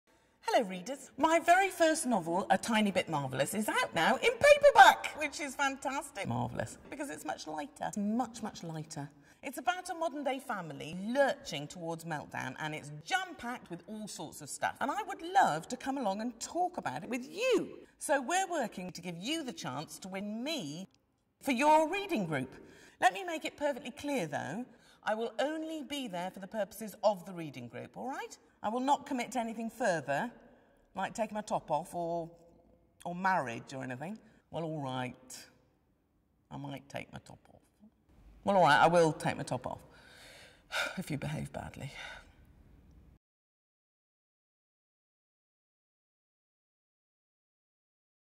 Dawn French at your Reading Group
In 2011 Penguin Books, the Booksellers Association and the Reading Agency celebrated the publication of A Tiny Bit Marvellous, the No.1 Bestseller from author Dawn French, with a competition to Win Dawn French at a reading group!